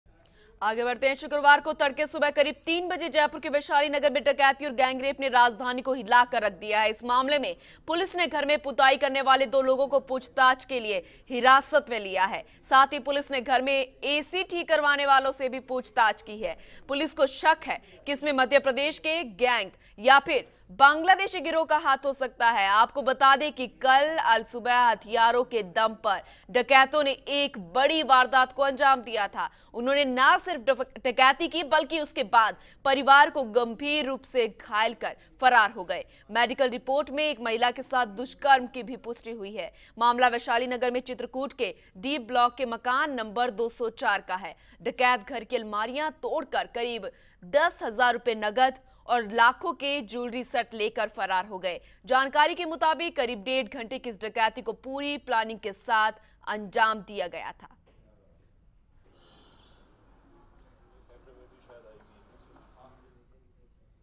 राजधानी जयपुर के चित्रकूट में लूट और रेप
शुक्रवार को तड़के सुबह करीब 3 बजे जयपुर के वैशाली नगर में डकैती और गैंगरेप ने राजधानी को हिला दिया है| इस मामले में पुलिस ने घर में पुताई करने वाले दो लोगों को पूछताछ के लिए हिरासत में लिया है| साथ ही पुलिस ने घर में एसी ठीक करवाने वालों से भी पूछताछ की है| पुलिस को शक है कि इसमें मध्यप्रदेश के गैंग या फिर बांग्लादेशी गिरोह का हाथ हो सकता है|

आपको बता दें कि कल अल सुबह हथियारों के दम पर डकैतों ने एक बड़ी वारदात को अंजाम दिया था| उन्होंने ना सिर्फ डकैती की बल्कि उसके बाद परिवार को गंभीर रुप से घायल कर फरार हो गए| मेडिकल रिपोर्ट में एक महिला के साथ दुष्कर्म की भी पुष्टी हुई है| मामला वैशाली नगर में चित्रकूट के D ब्लॉक के मकान नम्बर 204 का है| डकैत घर की अलमारियां तोड़कर करीब 10 हजार रुपये नगद और लाखों के ज्वेलरी सेट लेकर फरार हो गए| जानकारी के मुताबिक करीब डेढ घंटे की इस डकैती को पूरी प्लानिंग के साथ अंजाम दिया|